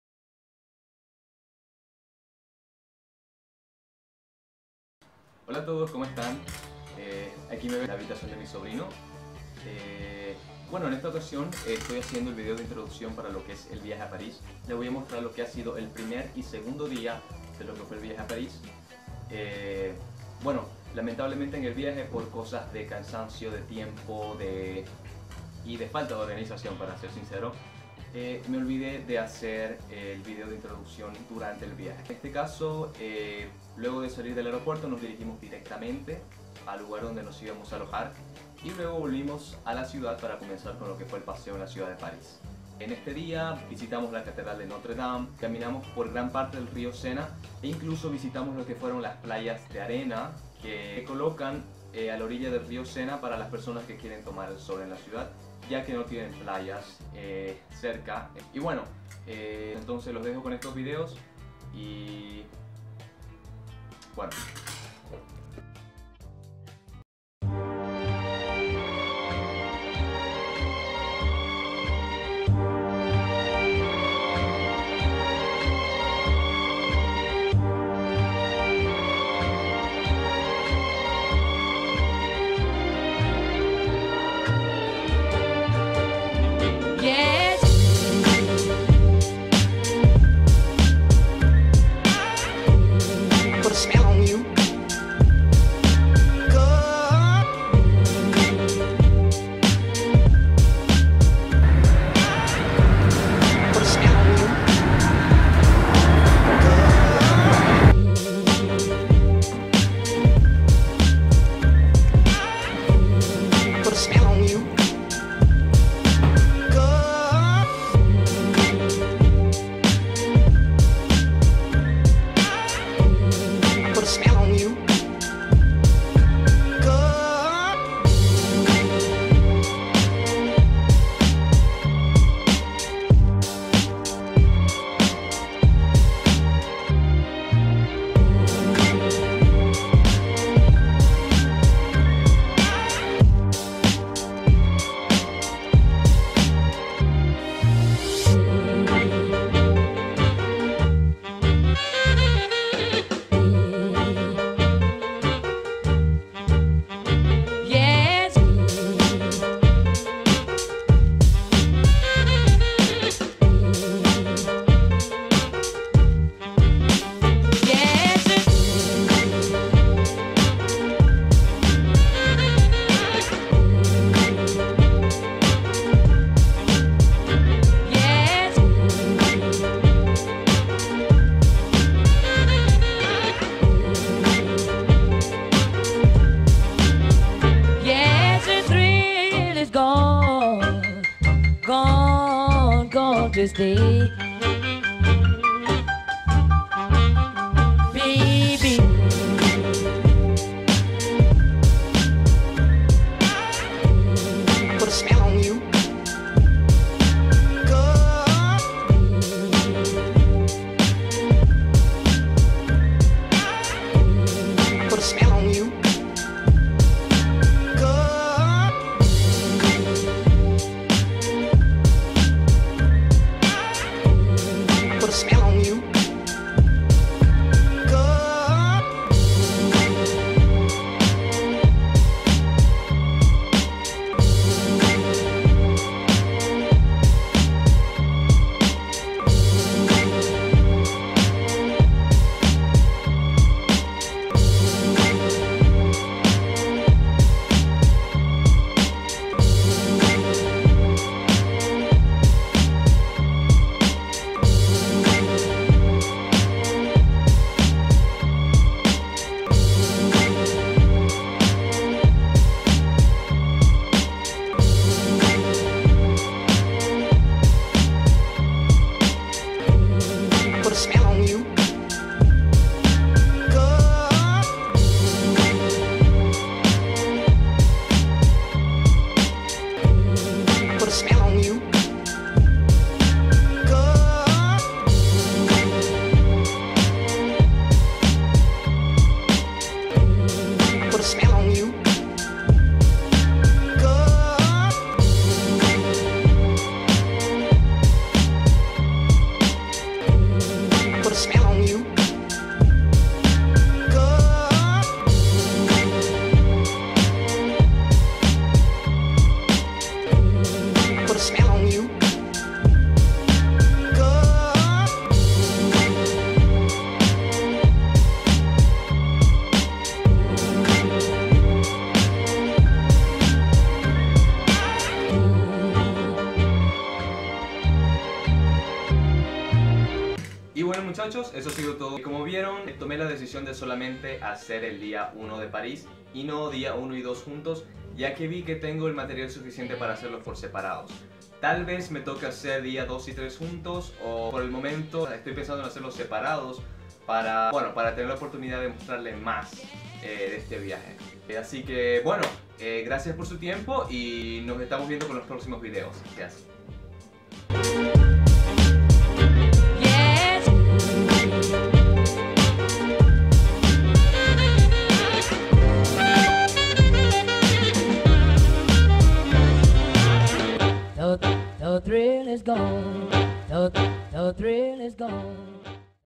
Conociendo París [Día 1] | #4 TRAVlog
Caminamos por las calles de París en nuestro primer día de visita.

Pasamos por lugares icónicos como la Catedral de Notre Dame, La gran rueda de París y el río Sena.

Increíbles vistas de los palacios y construcciones al mejor estilo francés.

Inspírate y visita esta maravillosa ciudad por tu cuenta!

Gracias!